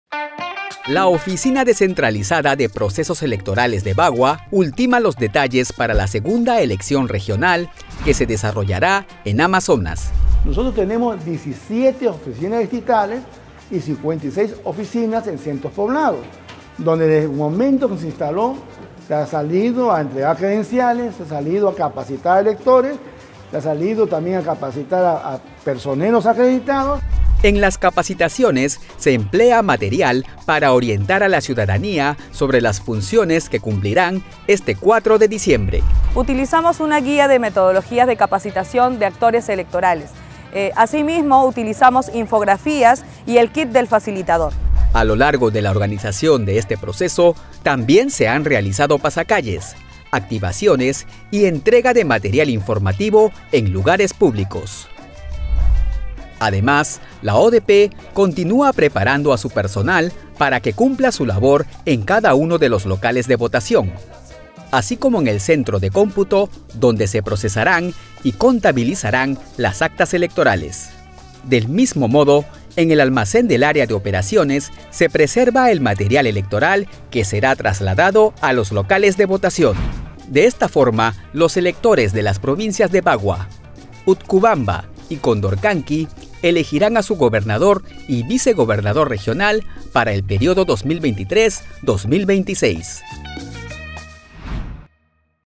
SER2022🗳️🇵🇪 | ¡Todo listo! para el 4 de diciembre en nuestra ODPE Bagua
Nuestra ODPE Bagua cuenta con 17 oficinas distritales y 56 en centros poblados para garantizar la organización y ejecución de la segunda elección regional este 4 de diciembre.

También puedes llamarnos a FONO Onpe: (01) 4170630 - Opción 1